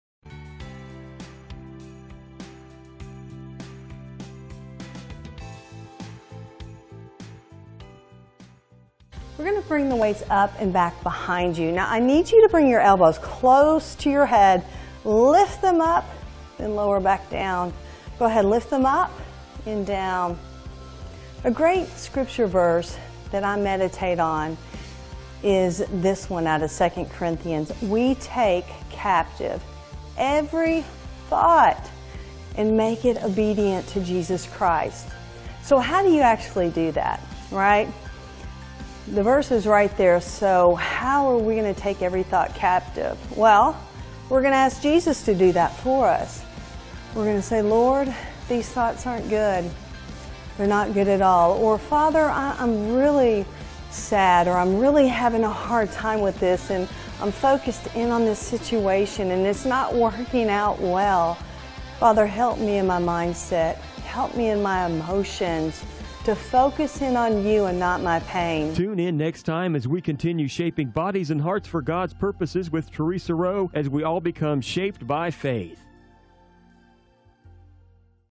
Triceps Extension Arm Strength Training Exercise with Scripture | Shaped by Faith TV | Theresa Rowe
Hope you'll give this quick exercise a try with me today! It is for every level of fitness so just go at your own pace. This verse is great to meditate on to help our mindset and to keep our focus on Jesus.

We take captive every thought to make it obedient to Christ.
2 Corinthians 10 : 5
Yes! Let's ask the Lord to help us with this as we shape our bodies and hearts for His purposes!

Shaped by Faith Scripture inspired Workout DVDs, Shirts, Devotional Book, Worship CDs and more are specially priced and available